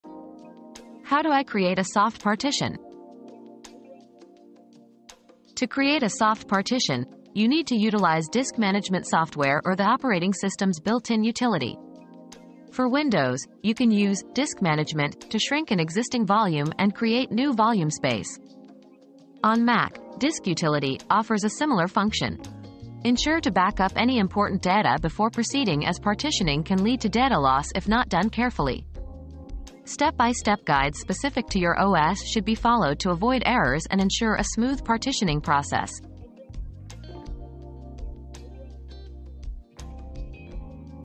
How do I create a soft partition?
Mastering Soft Partitioning: A Step-by-Step Guide • Soft Partitioning Guide • Learn how to create soft partitions on your computer using disk management software or built-in utilities. Follow step-by-step instructions for Windows and Mac to safely create new volume space without losing important data.